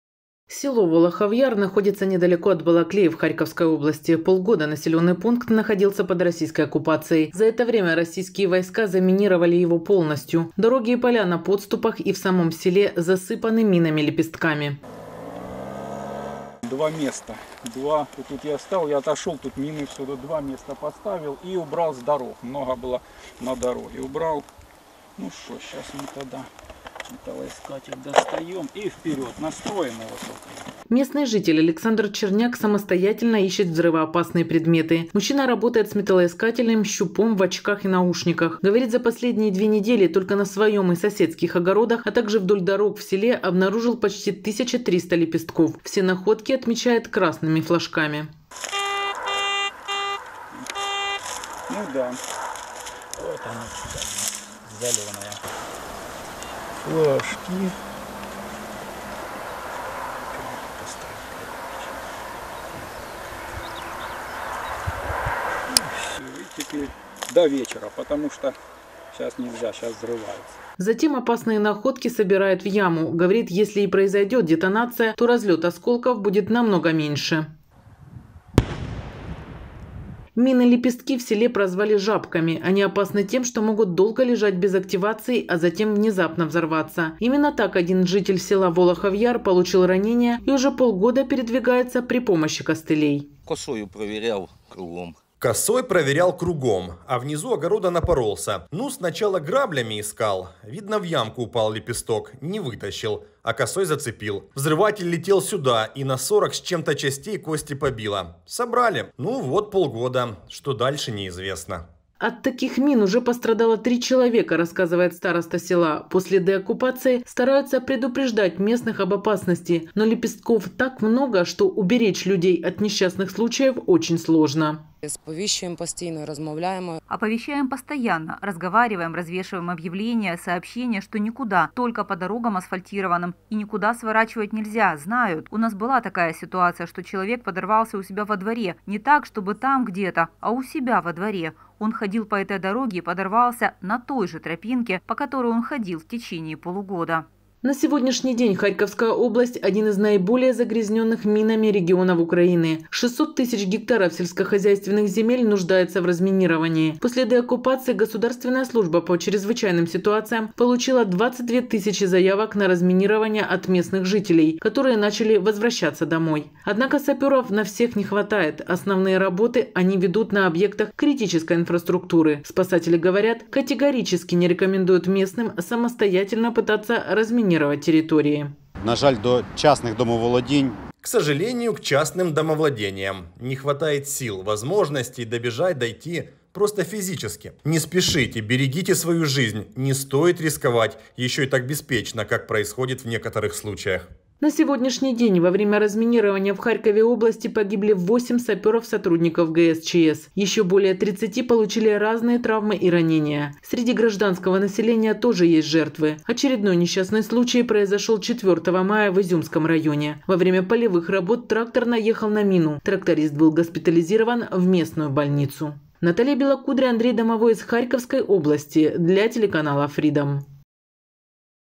Волохов Яр: как разминируют село под Балаклеей
125 тыс. саперов необходимо для разминирования Харьковской области. На сегодняшний день 30% территорий региона, которые были под российской оккупацией, и в местах, где велись активные боевые действия, – заминированы. Больше всего загрязнены взрывоопасными предметами север и юго-восток области. Территории расчищают 235 саперов со всей Украины – они выезжают на вызовы жителей населенных пунктов, а также занимаются разминированием объектов критической инфраструктуры. Одно из самых заминированных сел – Волохов Яр. Как там проходит разминирование – смотрите в материале.